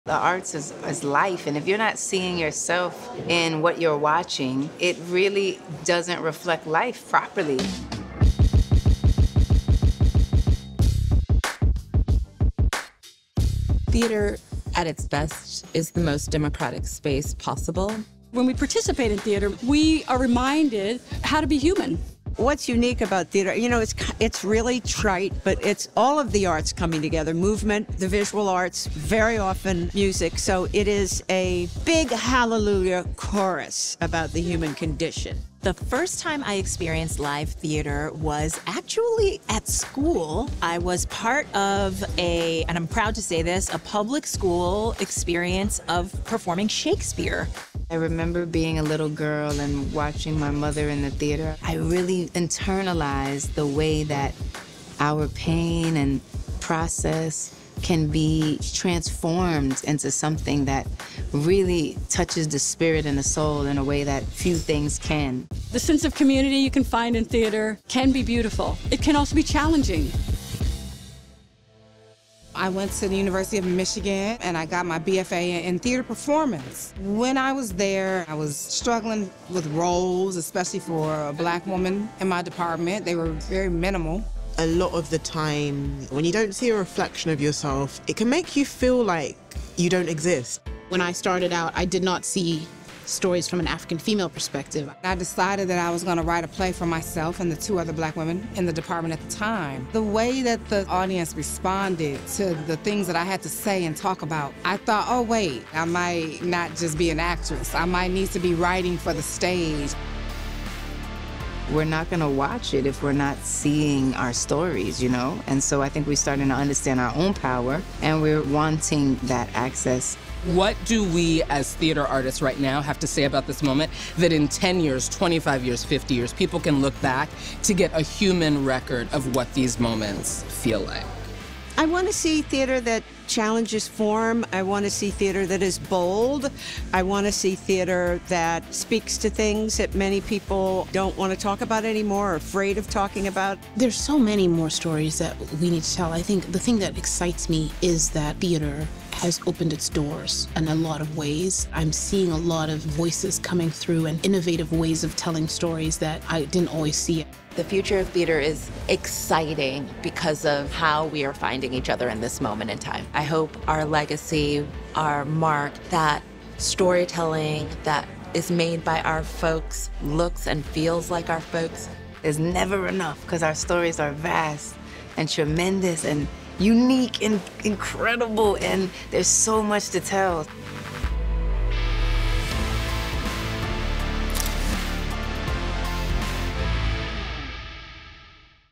The Storytellers Transforming The American Theatre | Harper's BAZAAR
American theatre is at the heart of entertainment, and these powerful faces are here to evolve the story of what theatre can be. Watch along as household names Alicia Keys, Sade Lythcott, Suzan-Lori Parks, Anna Deavere Smith, Sarah Jones, Dominique Morisseau, Jasmine Lee-Jones, Danai Gurira, Antoinette Chinonye Nwandu, and more talk about their experience growing within the theatre community. Listen to the stories of how theatre can be bold, speaks to topics that challenges form, and represents voices both on and off the stage.